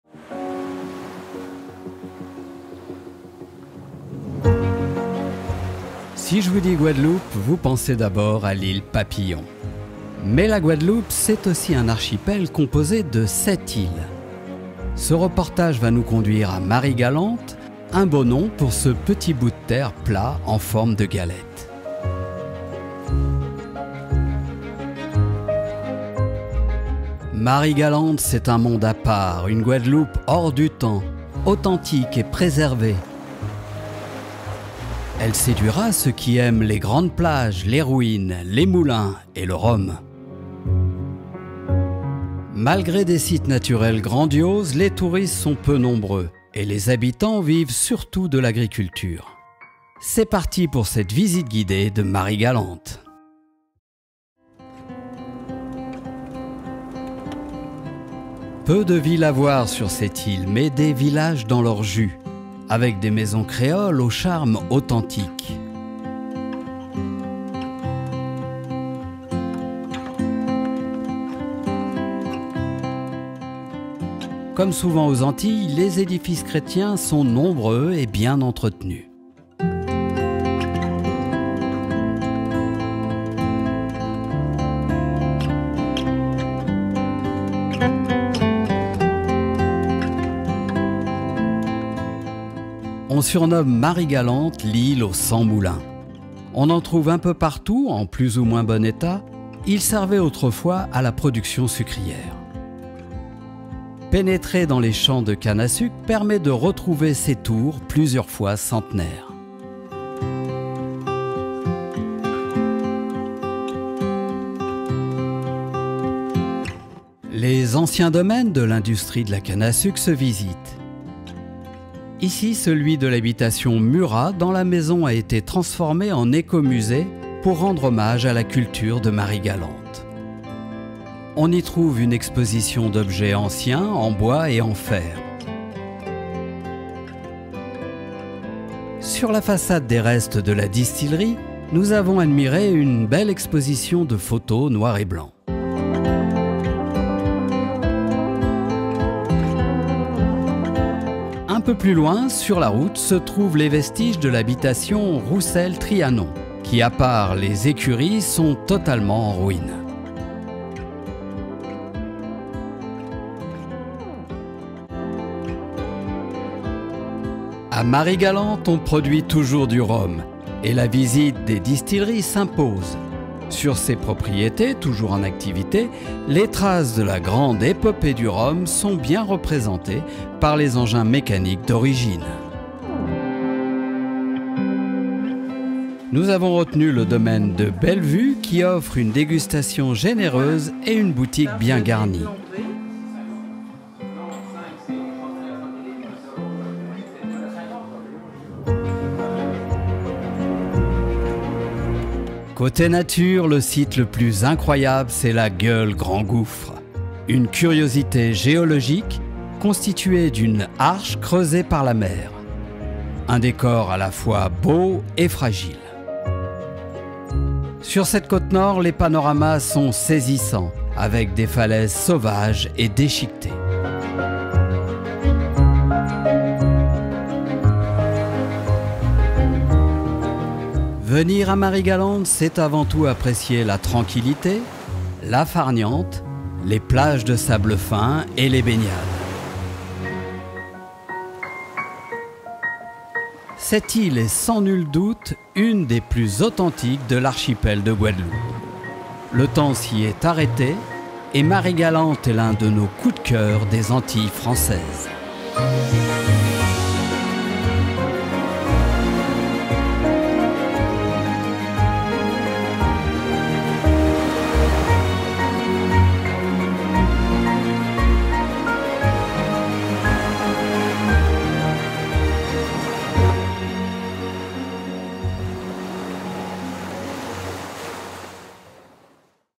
Marie Galante, visite guidée de l'île de Guadeloupe (4k)
Ce reportage nous conduit à Marie Galante, un beau nom pour ce petit bout de terre plat en forme de galette de l'archipel de Guadeloupe.
 Marie-Galante… C’est un monde à part, une Guadeloupe hors du temps, authentique et préservée.  Elle séduira ceux qui aiment les grandes plages,  les ruines, les moulins et le rhum.
Des visites, plutôt hors des sentiers battus, que nous partageons avec plaisir.